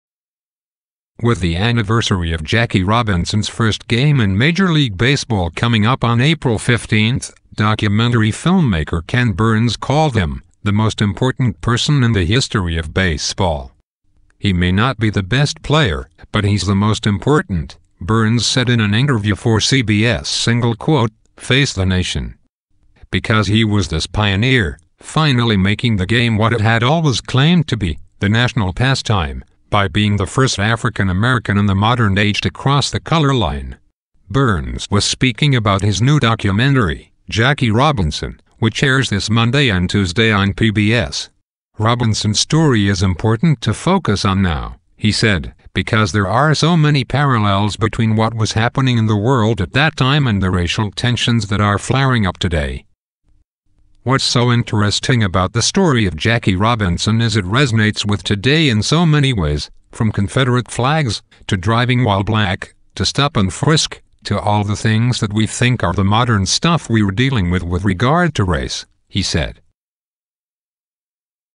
Ken Burns traces life of baseball great Jackie Robinson
With the anniversary of Jackie Robinson's first game in Major League Baseball coming up on April 15, documentary filmmaker Ken Burns called him "the most important person in the history of baseball."